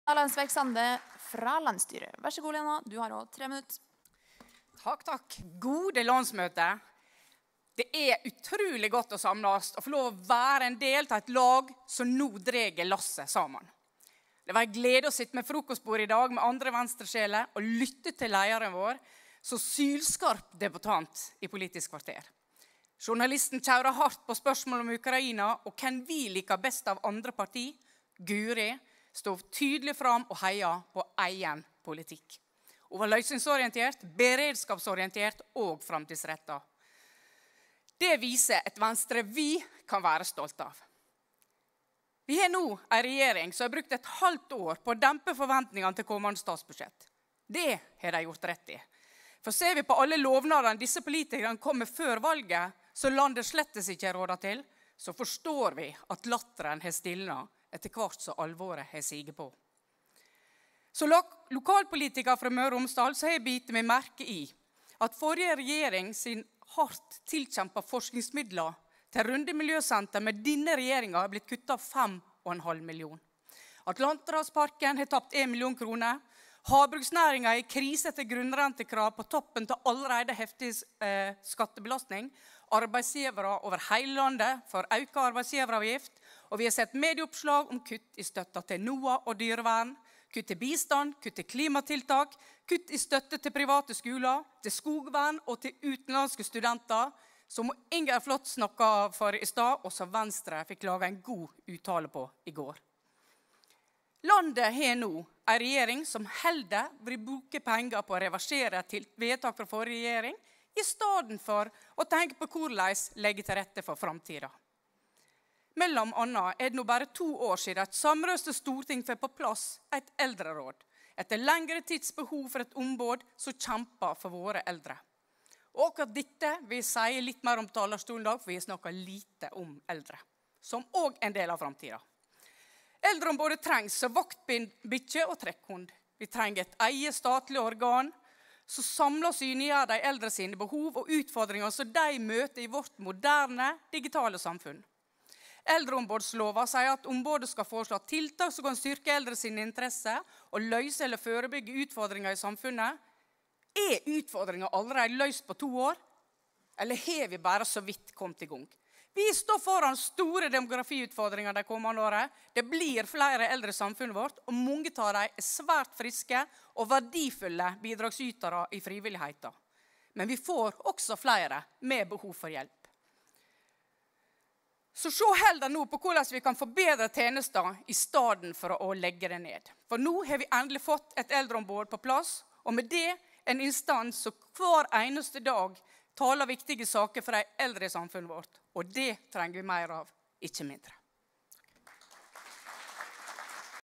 Lena Landsverk Sande (Landsstyret) - Venstres landsmøte 2022 (GPD)
Innlegg i "Generell politisk debatt" på Venstres landsmøte 2022 (Gardermoen) En Venstre-video